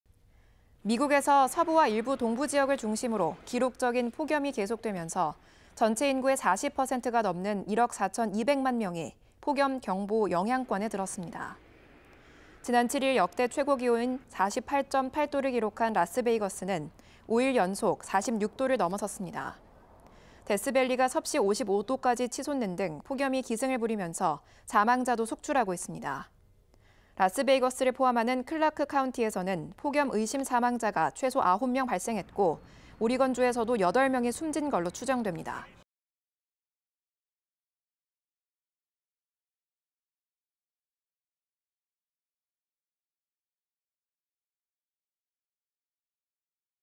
라스베이거스 5일 연속 46도 넘어... 美 기록적 폭염에 사망자 속출
미국에서 서부와 일부 동부 지역을 중심으로 기록적인 폭염이 계속되면서 전체 인구의 40%가 넘는 1억4천200만 명이 폭염 경보 영향권에 들었습니다.

지난 7일 역대 최고기온인 48.8도를 기록한 라스베이거스는 5일 연속 46도를 넘어섰습니다.

데스밸리가 섭씨 55도까지 치솟는 등 폭염이 기승을 부리면서 사망자도 속출하고 있습니다.

라스베이거스를 포함하는 클라크 카운티에서는 폭염 의심 사망자가 최소 9명 발생했고, 오리건주에서도 8명이 숨진 것으로 추정됩니다.